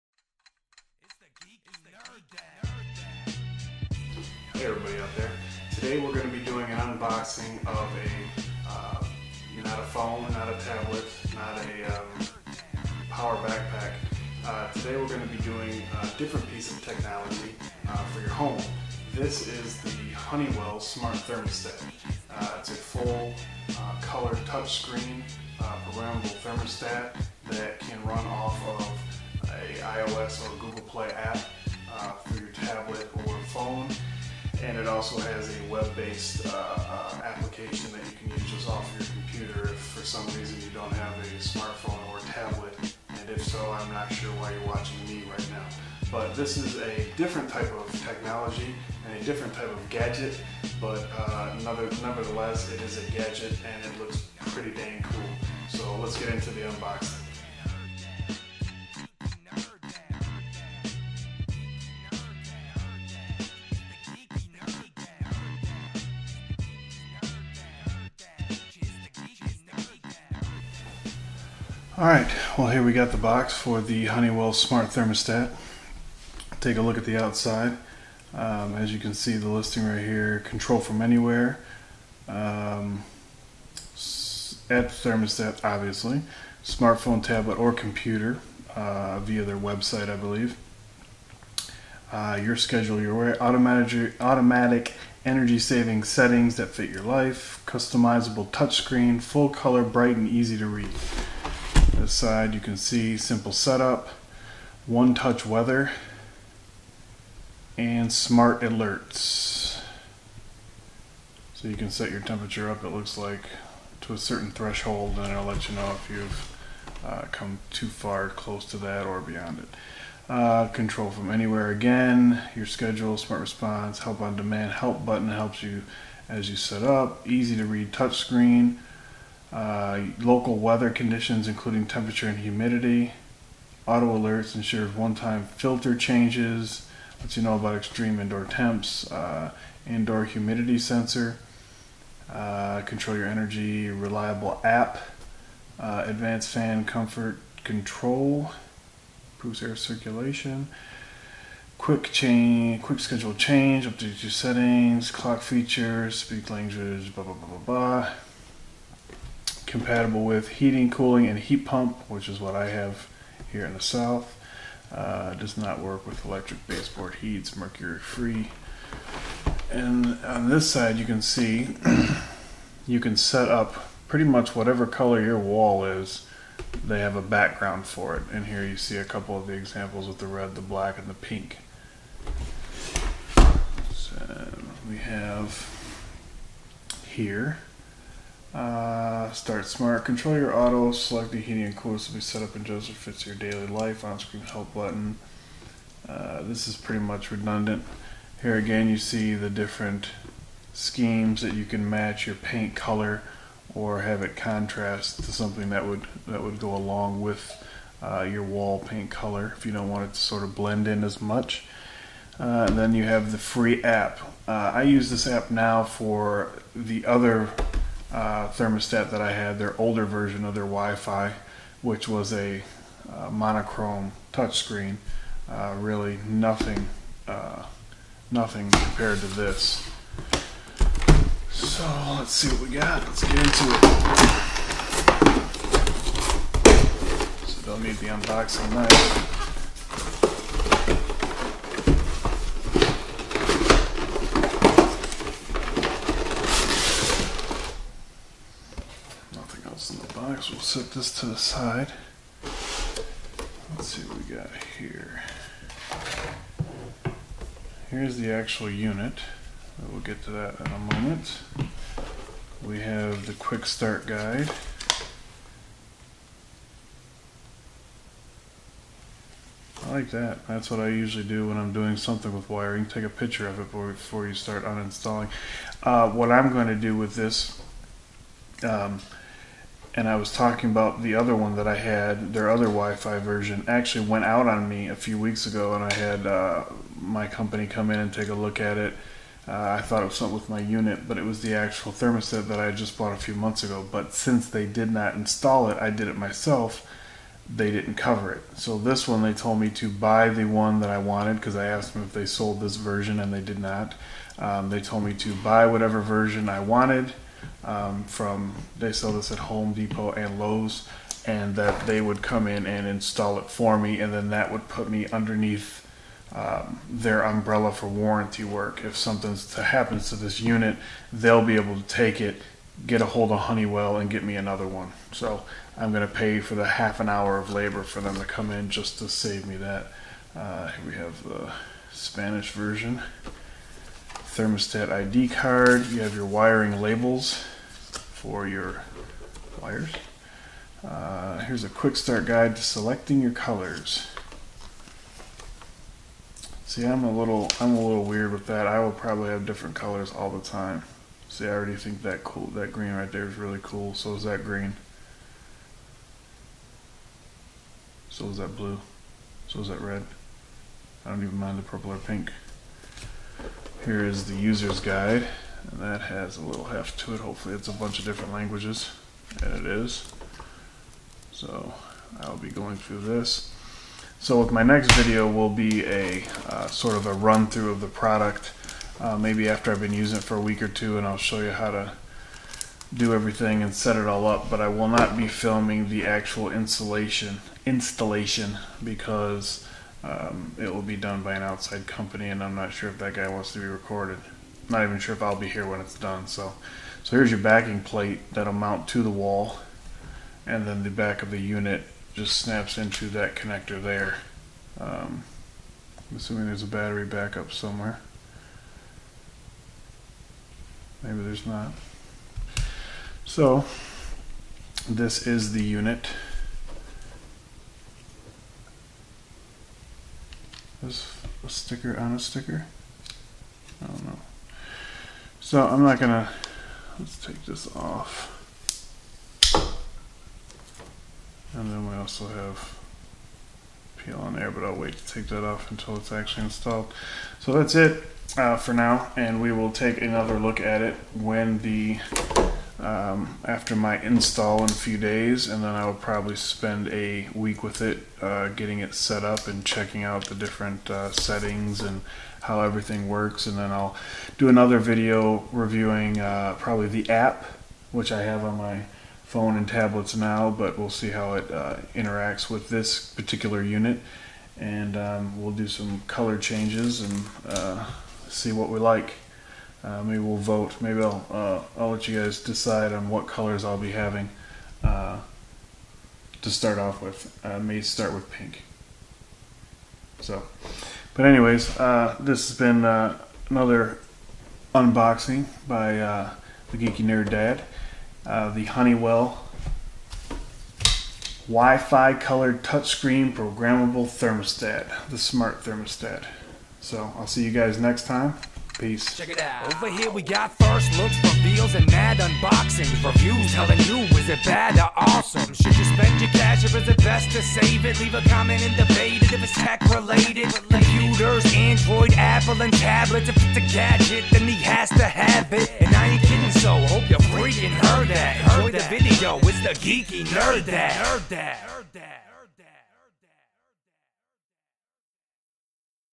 Unboxing- Honeywell Smart Thermostat Model # RTH9580WF
Today we unbox the NEW Honeywell Smart Thermostat Model # RTH9580WF

Songs by Grover check him out on

You Tube- GroverGrove1